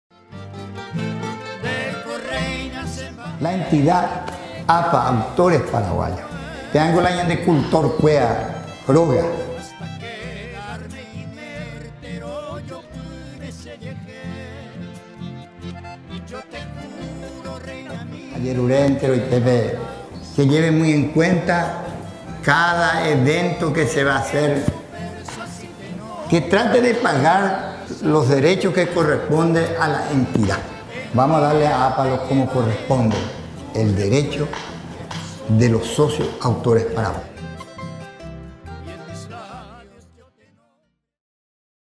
Néstor Damián Girett – Mensaje a la Gente.
Mensaje de Néstor Damián Girett a la Gente.

“….La entidad APA, Autores Paraguayos…es la casa de nuestros cultores.

Les pido a todos que lleven muy en cuenta, cada evento que se va a hacer, que trate de pagar los derechos que corresponden a la entidad, vamos a darle a APA como corresponde, el derecho de los socios autores paraguayos.”